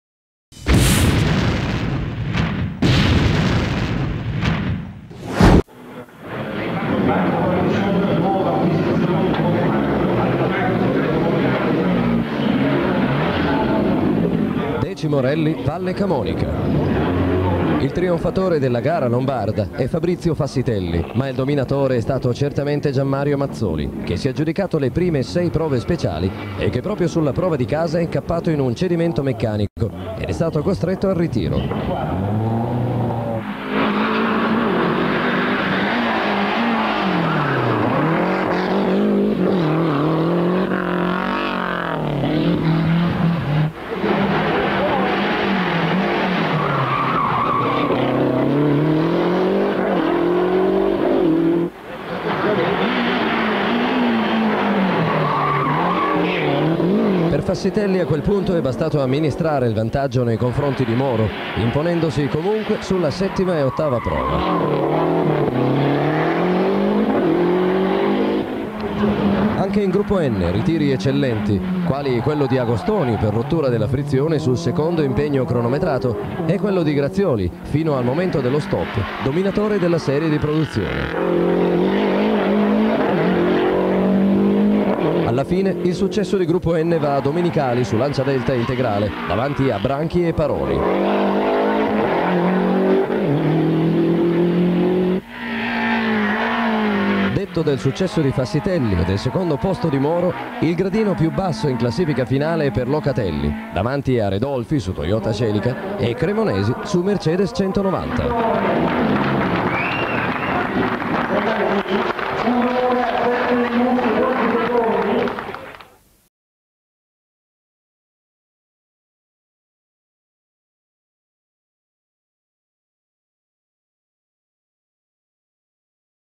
10' Rally Valle Camonica (sintesi)
Breve servizio Tv su questa edizione dominata da Mazzoli su Lancia Delta Gr A che nelle battute finali veniva fermato da un cedimento meccanico. Vinceva così la gara Fassitelli su analoga vettura.